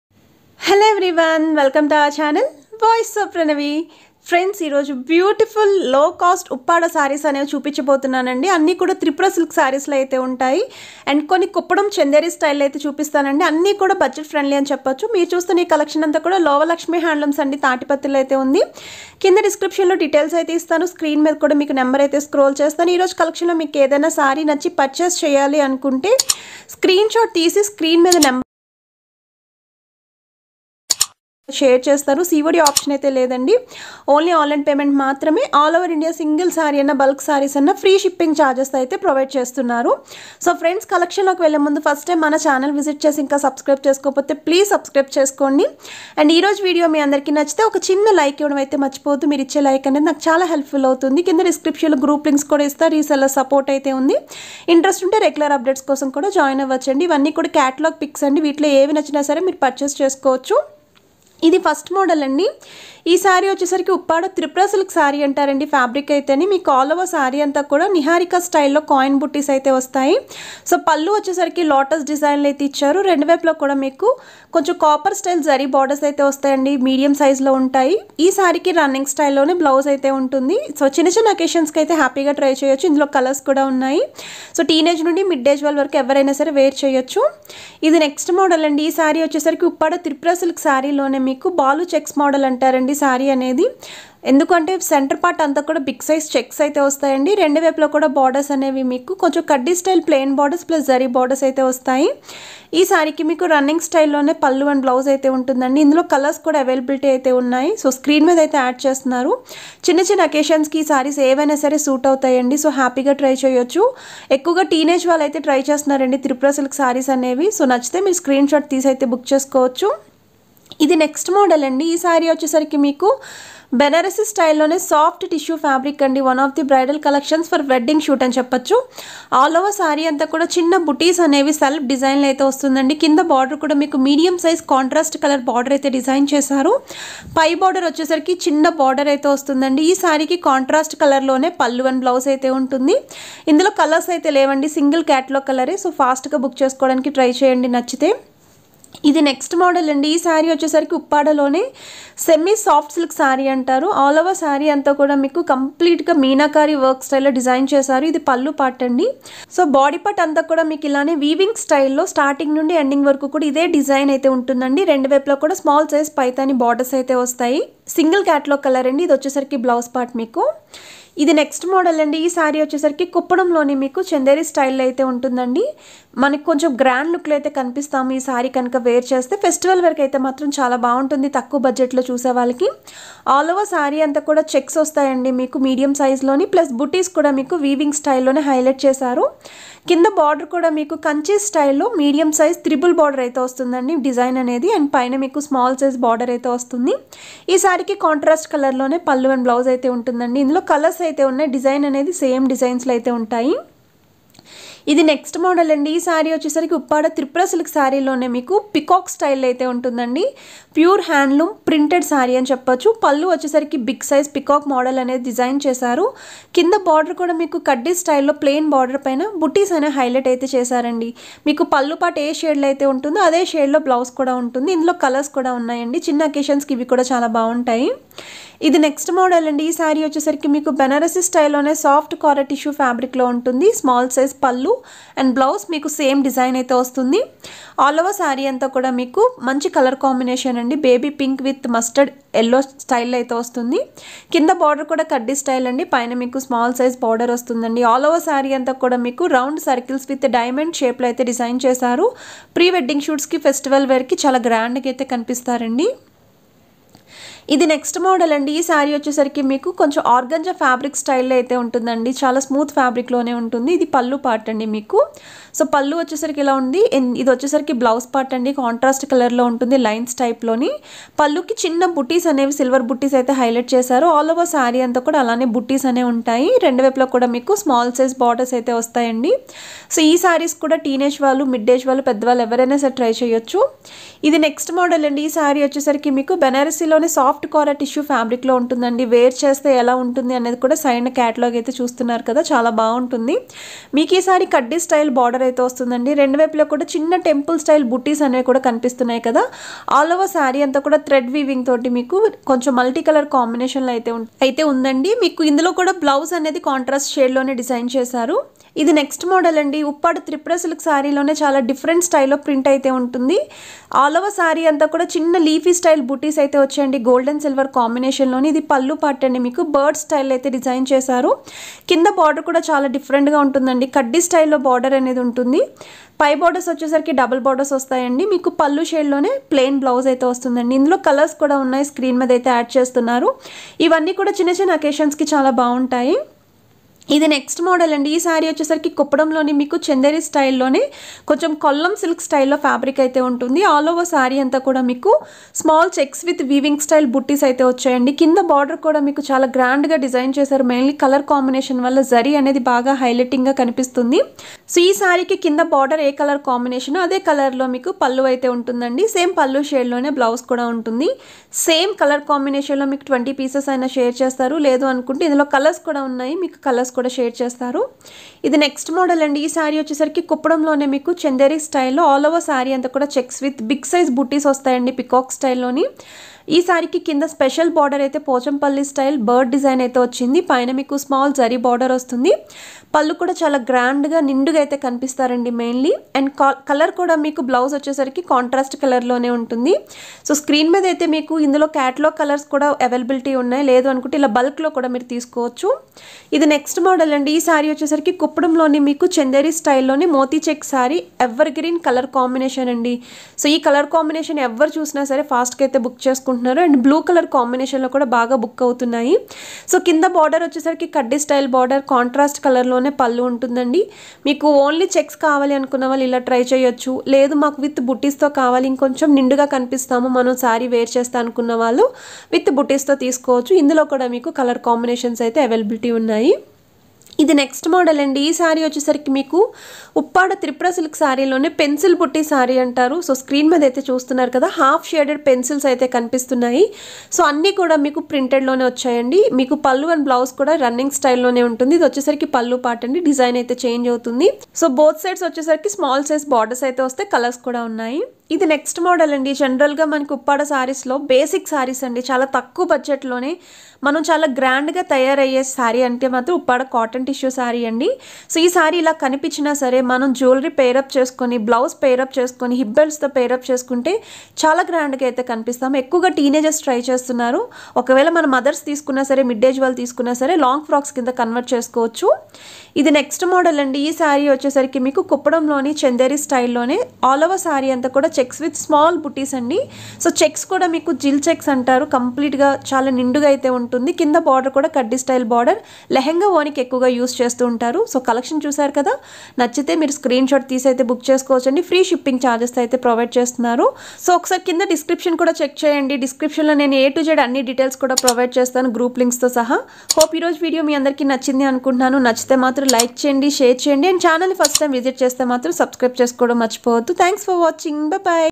సంక్రాంతి పండుగ సందర్భంగా తక్కువ బడ్జెట్ గ్రాండ్ లుక్ తెచ్చే అందమైన ఉప్పాడ కుప్పడం బెనారస్ శారీస్
Address :
Lova Lakshmi Handlooms
Tatiparthi
Gollaprolu Mandal
East Godavari District
Andhra Pradesh
Pin : 533445